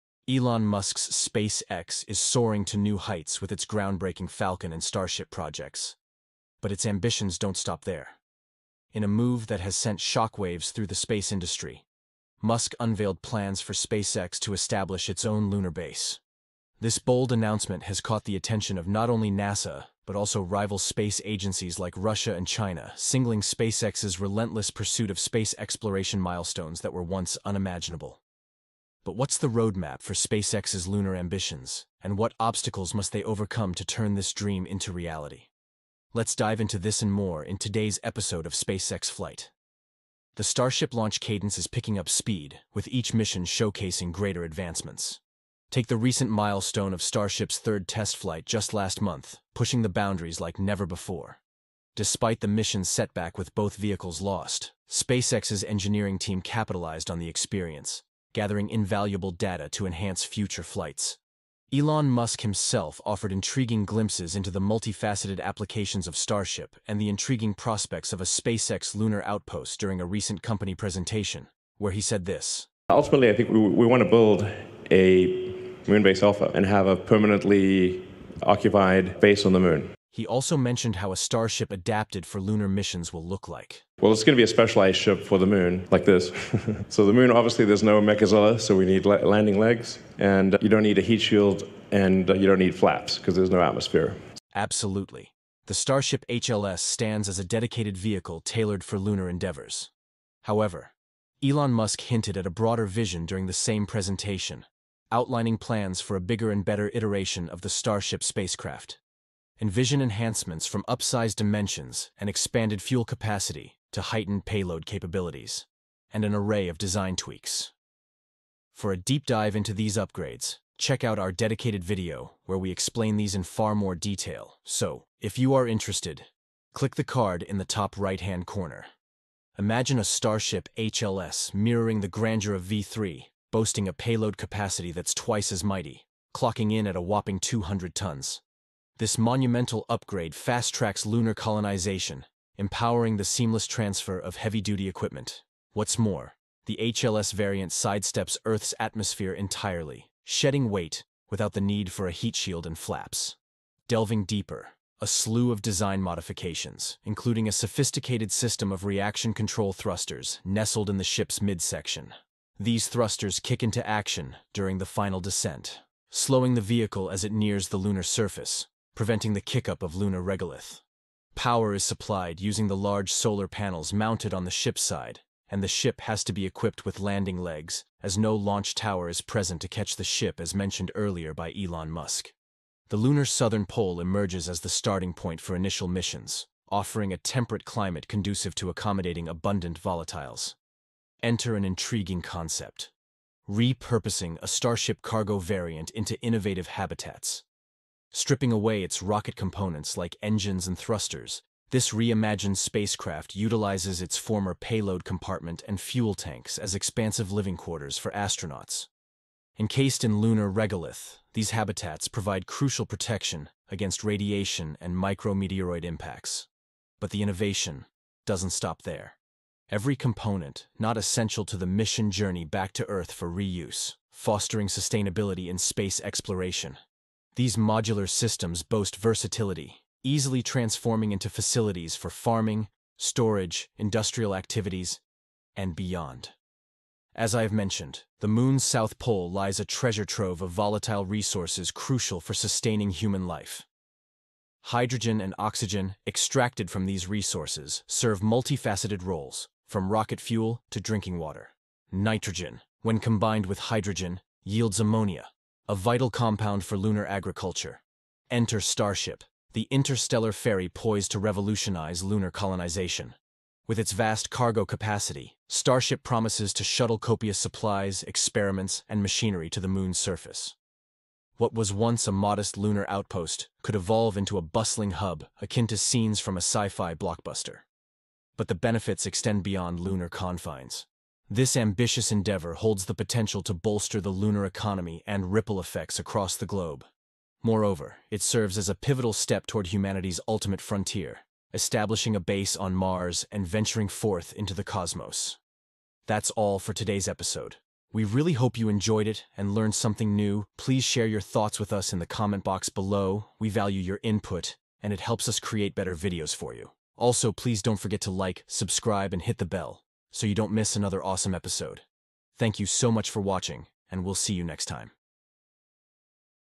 SpaceX's Mind Blowing Moon Colonisation Plan Unveiled: Inside SpaceX's Alpha Moonbase Revolution!
Get ready for the latest scoop in space exploration! In this video, we dive deep into the groundbreaking updates on SpaceX's Starship lunar program. Discover the exciting details of the newly announced "Alpha Moonbase" and how SpaceX's innovative design upgrades are revolutionizing the future of lunar colonization.

Elon Musk's SpaceX is on a mission to make history, and with the unveiling of their ambitious lunar plans, the space race has reached new heights. Learn about the cutting-edge features of the upgraded Starship spacecraft, poised to lead humanity's journey to the moon and beyond.

But that's not all – we'll also explore how SpaceX's rivals are struggling to keep pace with the groundbreaking advancements, setting the stage for an epic showdown in the race to space dominance.

Don't miss out on this thrilling update that promises to reshape the future of space exploration! 🌕✨